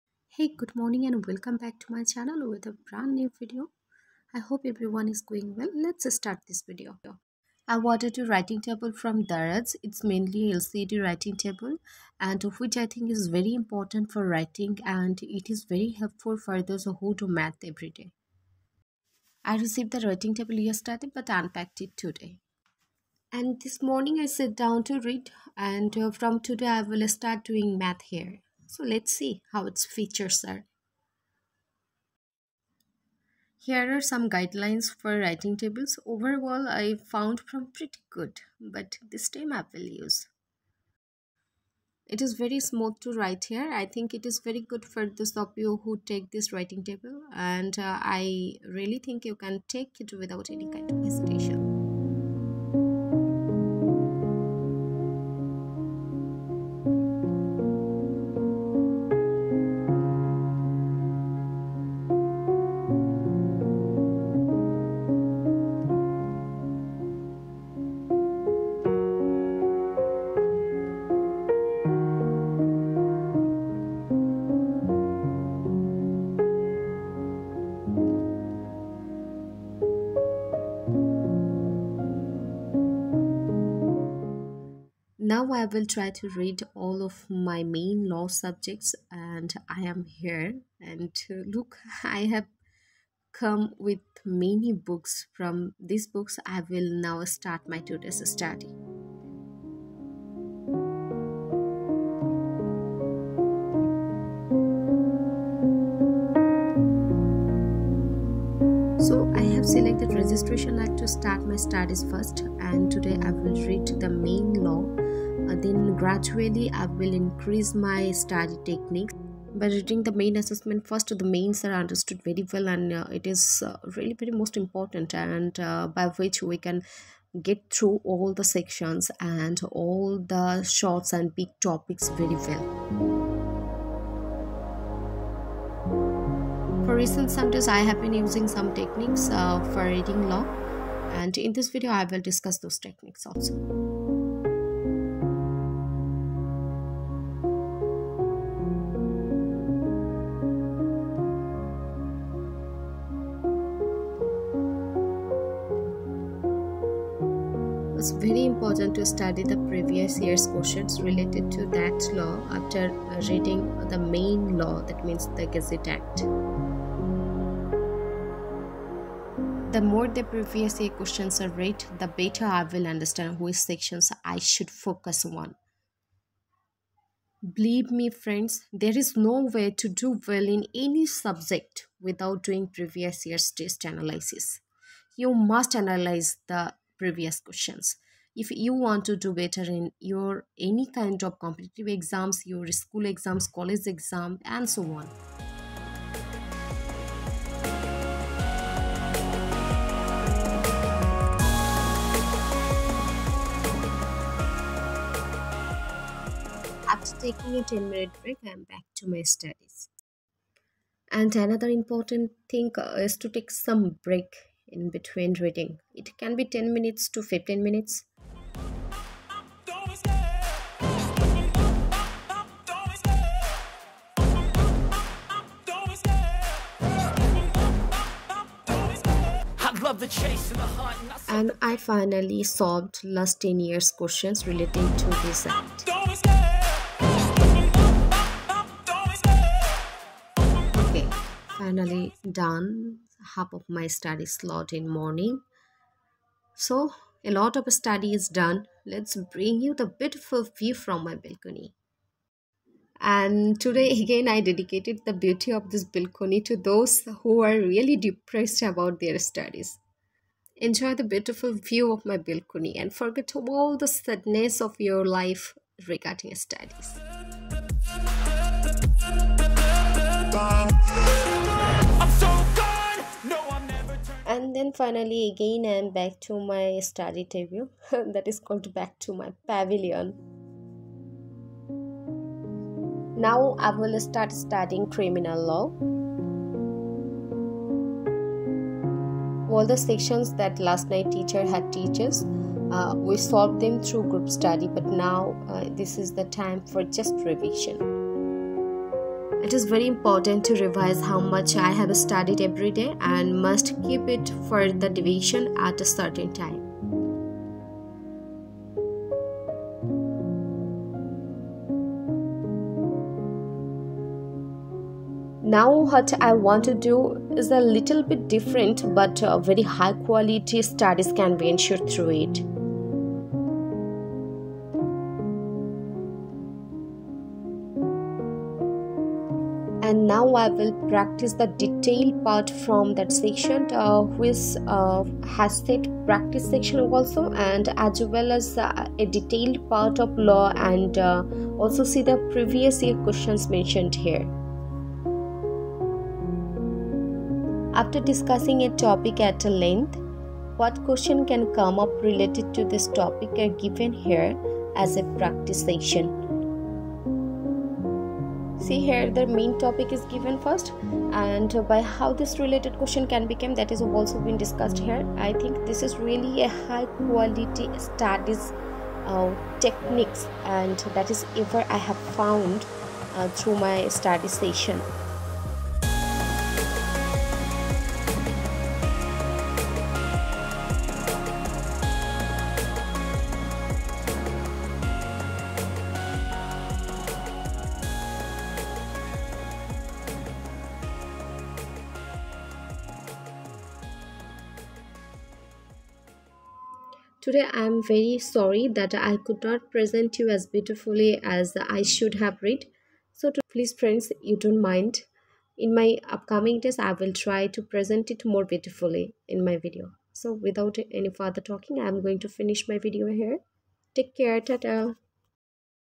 My Productive morning study vlog. 6 am to 2 pm
I hope everyone is pretty well and doing his preparation very productively and obviously with the continued process.  today in my blog I share my 6:00 to 2:00 p.m. study routine. and also share here are some of my Study techniques regarding law. I hope you guys really enjoyed this video and I hope this video will be very helpful to you all. thank you so much.